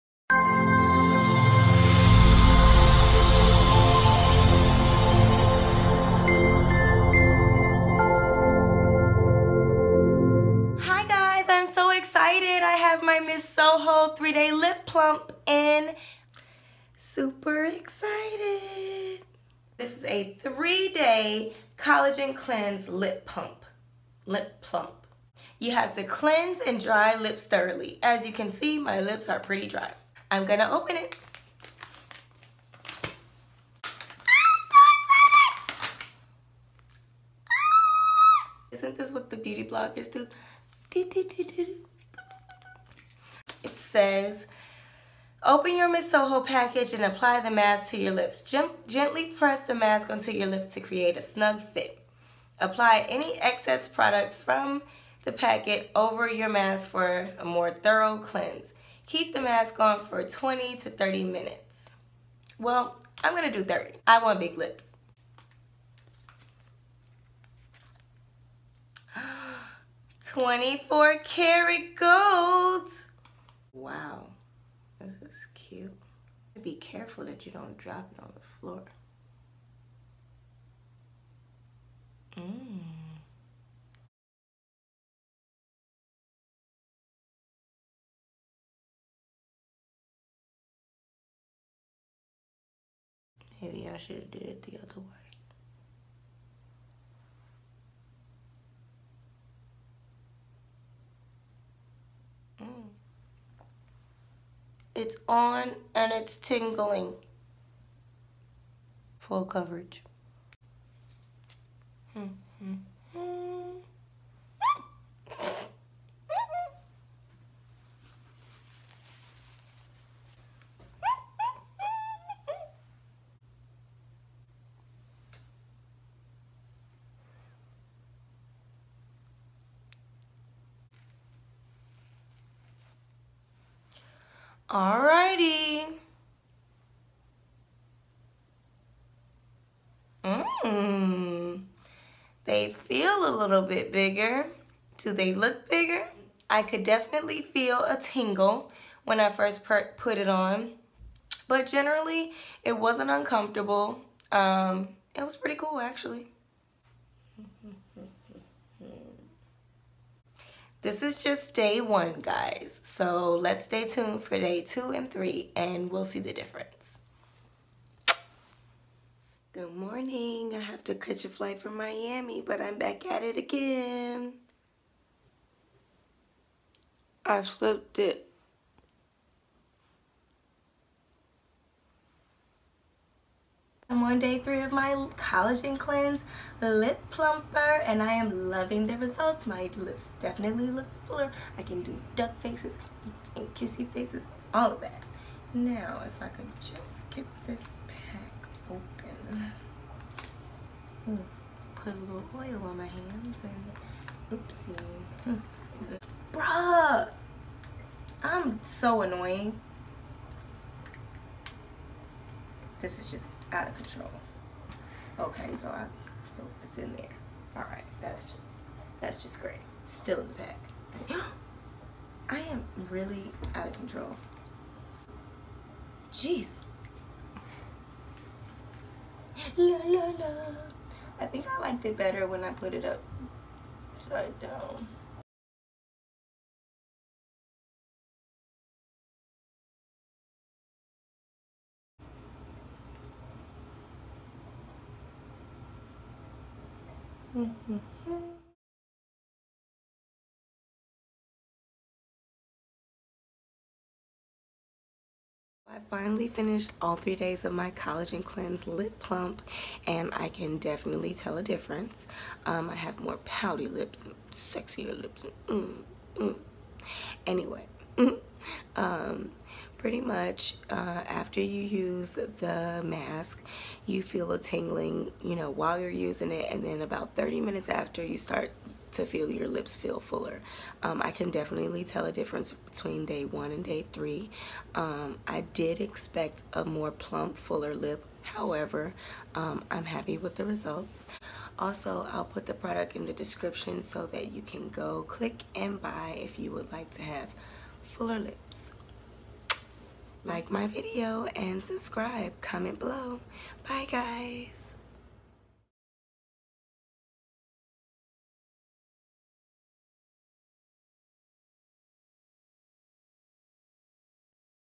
Does It Really Work? Ms. Soho 24k Collagen Lip Plump Review
I tried the Ms. Soho Collagen Cleanse (three day lip plump).  It was $24.95.  When you put it on, it tingles; then the tingles fade away. I kept my mask on for 30 minutes.  I wanted the plumpest lips possible.  After the lip plump mask was off I could feel a little tingle 30 minutes after.

Yes.  But i expected more plump lips.  I could definitely tell the difference though.

Directions for use of each mask:

1. Cleanse and dry lips thoroughly.

2. Open your Ms. Soho package and apply the mask to your lips.

3. Gently press the mask onto your lips to create a snug fit.

4. Apply any excess product from the packet over your mask for a more thorough cleanse.

5. Keep the mask on for 20 to 30 minutes.

6. Remove the mask gently, and rub in any excess moisture into the lips.